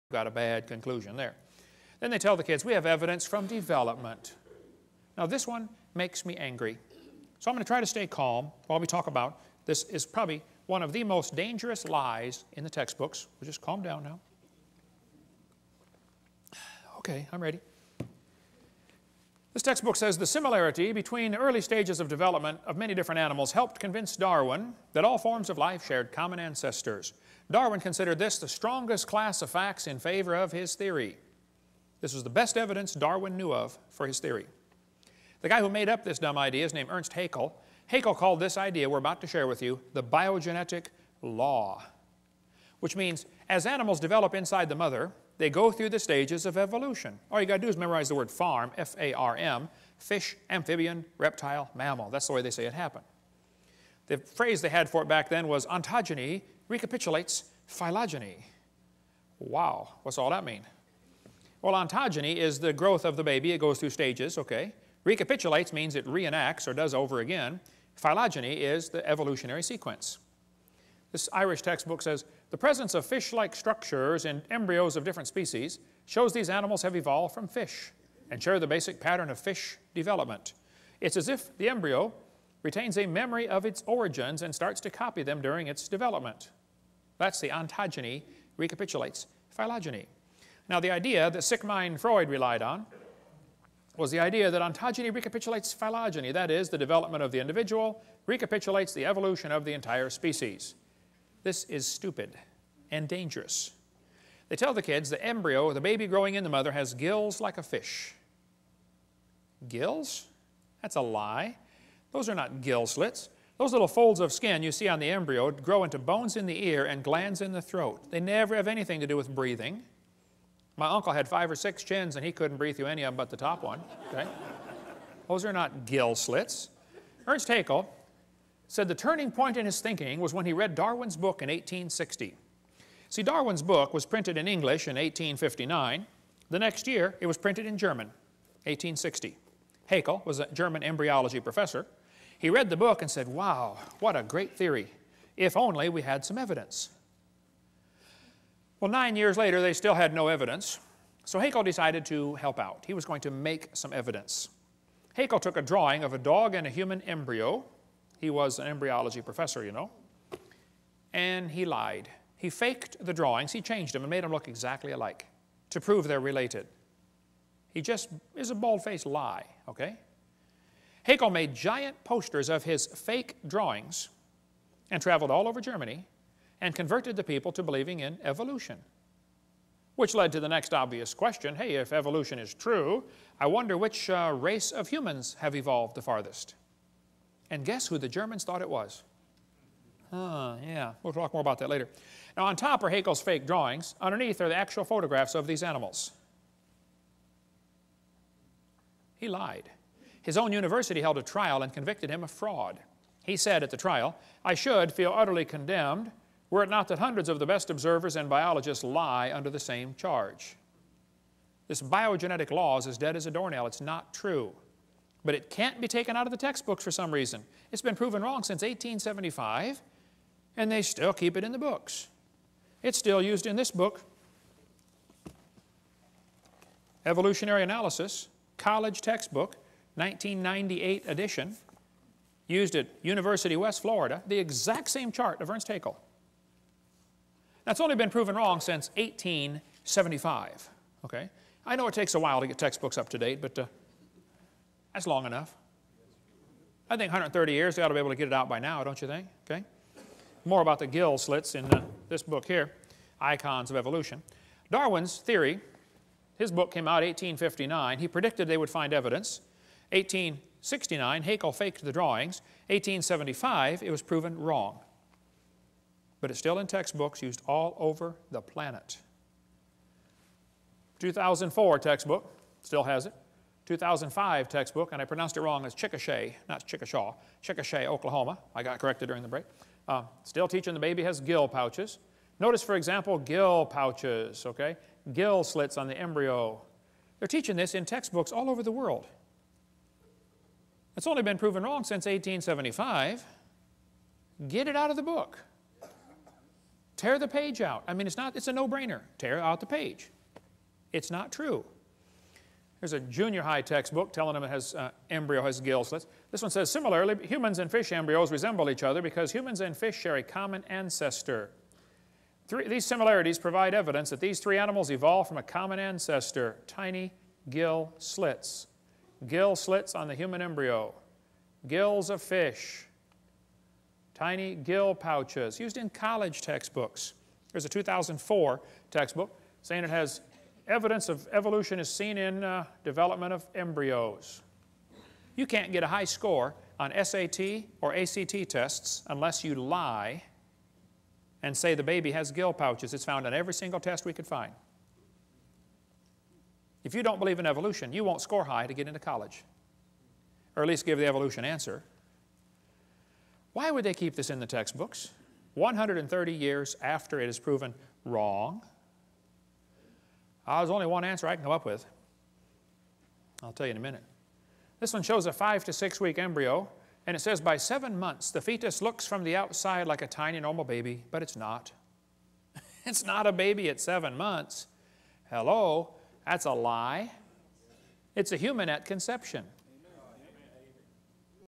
26. Earnst Haeckel . Biogenetic Law . Ontogeny Recapitulates Phylogeny (DVD 4, 1hr10:31-1hr16:52)
26. Evidence From Development . Darwins Best Evidence . Earnst Haeckel . Biogenetic Law . FARM . Fish . Amphibian . Reptile . Mammal . Ontogeny Recapitulates Phylogeny . Fraud Fake . Drawings .  Proven Wrong since 1875 . Abortion (DVD 4, 1hr10:31-1hr16:52)